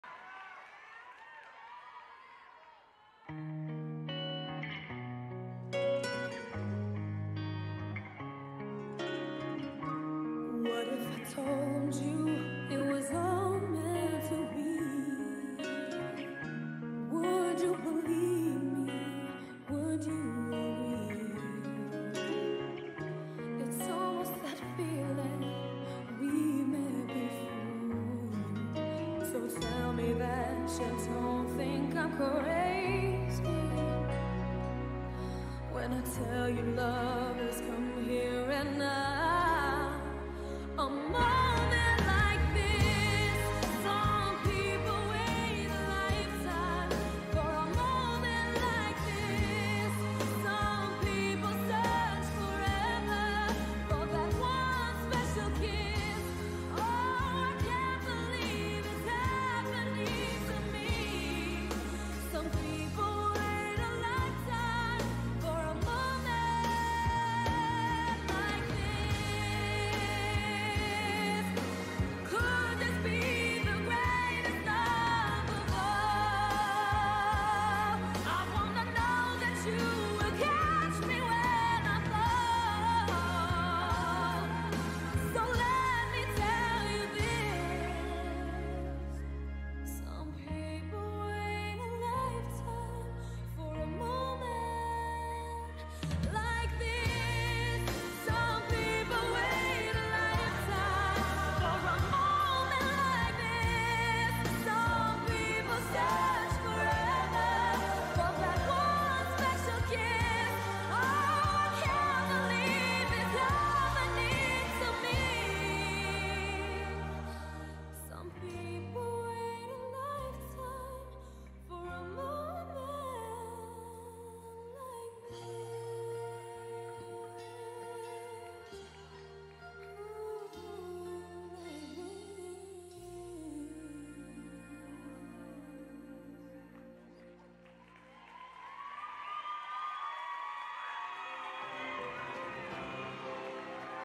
Belle Marie Arauz - A Moment *PETITE MISS WORLD DANCE*
Belle Marie recompetes her solo for the title of Petite Miss World Dance at the 2022 World Dance Pageant!